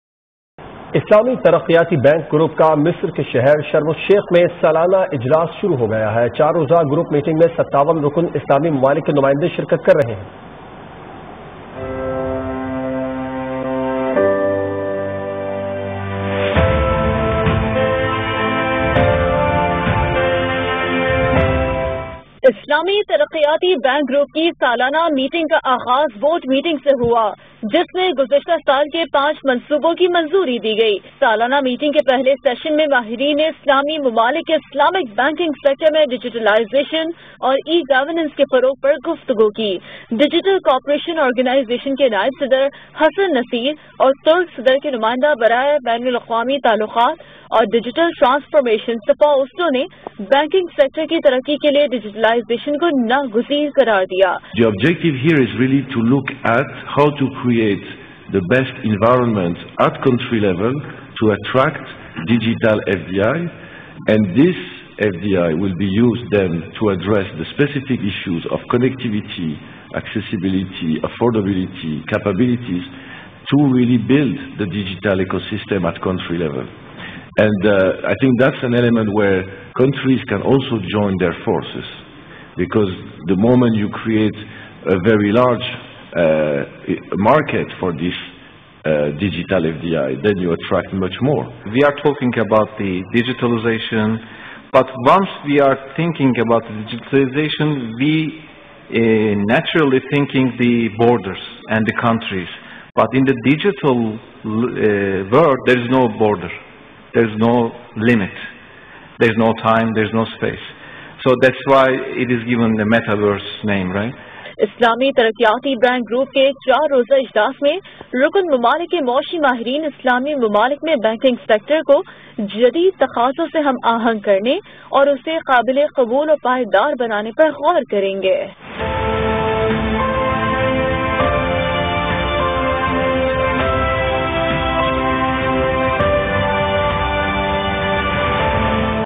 The annual meeting of the Islamic Development Bank (IsDB) Group started in Egypt's Sharm el-Sheikh
The four-day meeting under the main theme of "Beyond Recovery: Resilience and Sustainability" is being attended by ministers from 57 member countries and representatives of Islamic banks and financial institutions.
"The meeting will represent an opportunity to study and explore ways to further consolidate economic relations amongst IsDB Group member countries in today's changing world," the bank said.